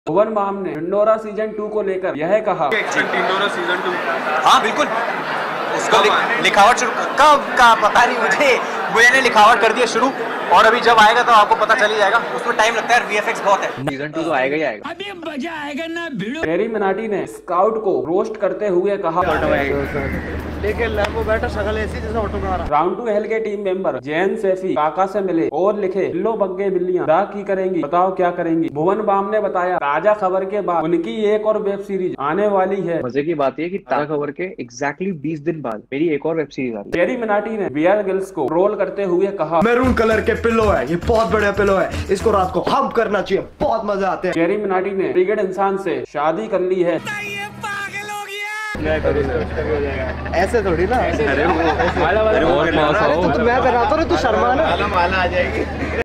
talks about dhindora session 2,@Round2hell, @CarryMinati,@triggeredinsaan,@officialscoutop
@BBKiVines  talks about dhindora session 2,@Round2hell , @CarryMinati ,‎@triggeredinsaan ,‎@sc0utOP

buy -

main mic

boya mic

room light

about this video - es video aapke liye bhut saari youtuber news our facts hai.

00:00 bb ki vines talks about dhindora session 2
00:13 carryminati roast scout
00:22 round2hell team member zayn saifi meet kaka
00:29 bb ki vines talks about new web series after taaza khabar
00:37 carryminati troll bear grylls
00:44 carryminati marriage with triggered insaan